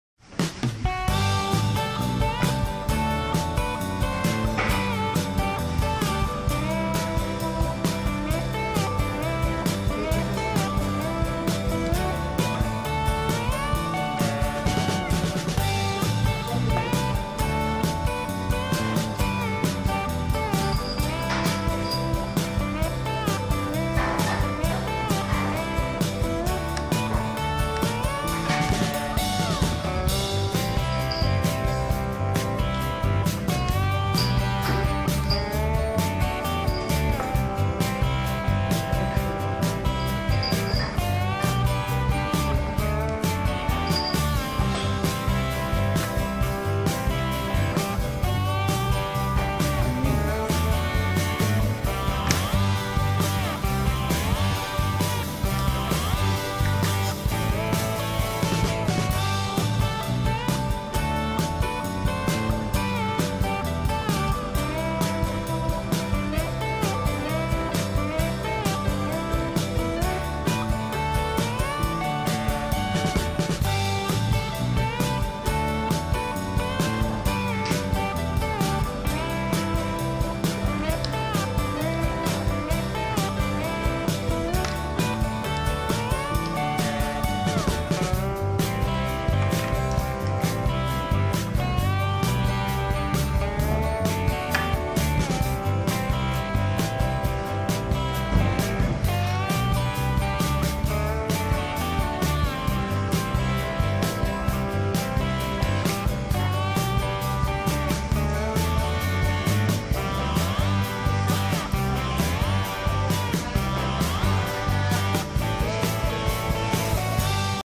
受講生の菊練り
ゼロからはじめてここまで出来るように指導しています。
まどかぴあ木曜やきもの講座　福岡県大野城市